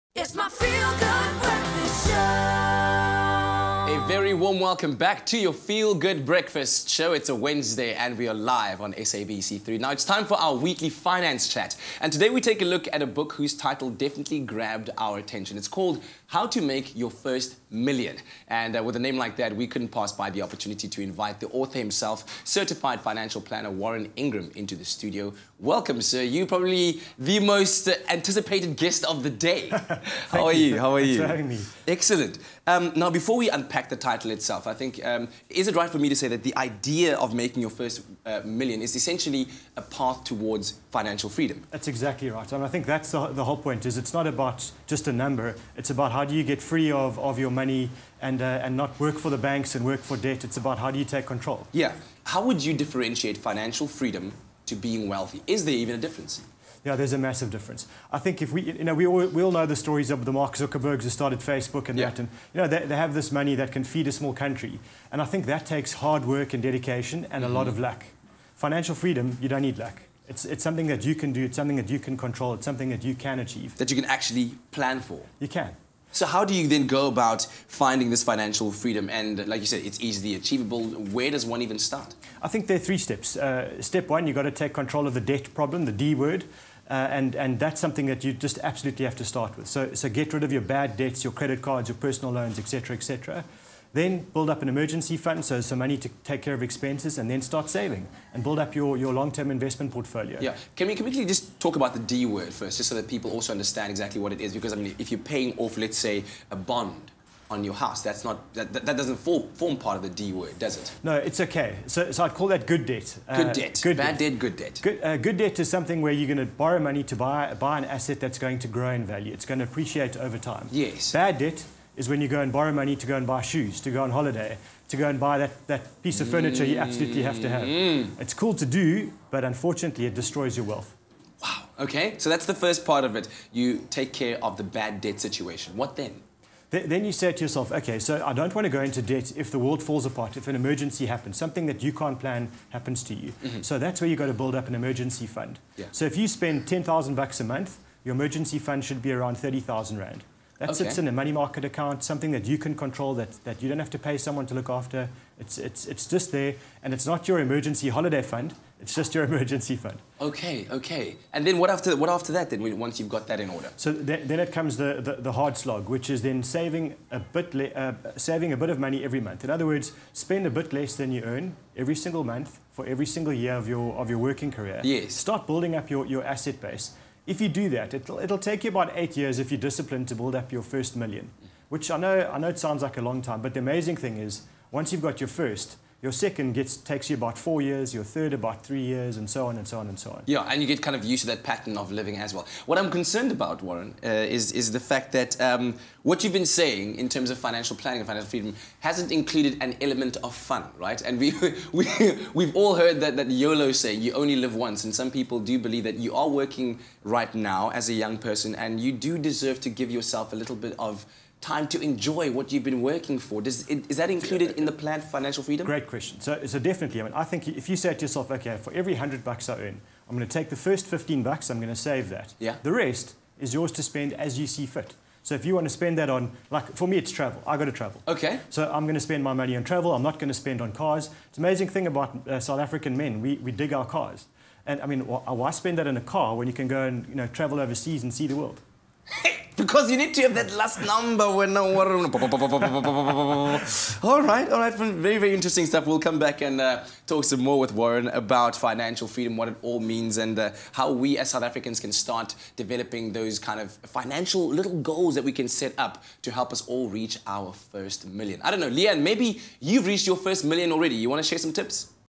Finance – How To Make Your First Million Part 1
In today’s finance segment we’re joined by Warren Ingram, financial planner and author of “How to Make Your First Million”.